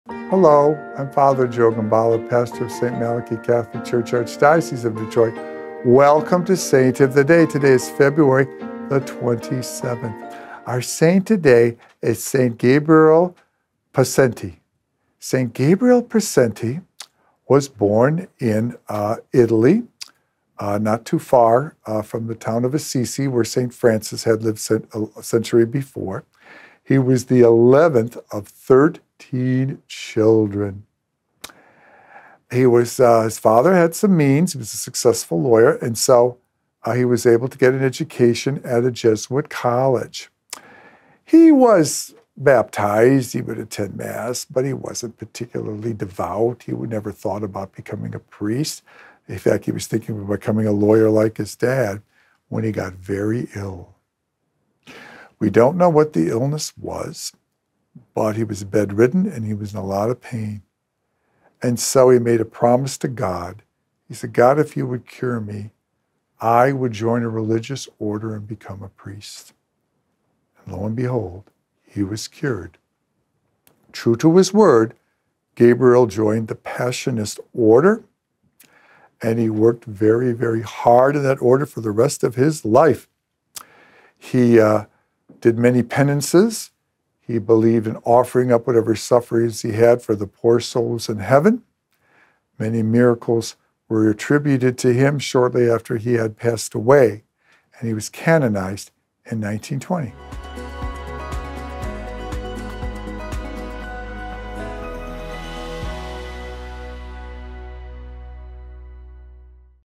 St. Gabriel Possenti
St. Gabriel Possenti was born in Italy and was the 11th out of 13 children. He able to get an education, but became very ill. He made a promise to God to become a priest if he was healed. He was cured!

Our Saint of the Day videos are only possible due to the contribution of viewers like you - Prayerfully consider financially supporting our ministry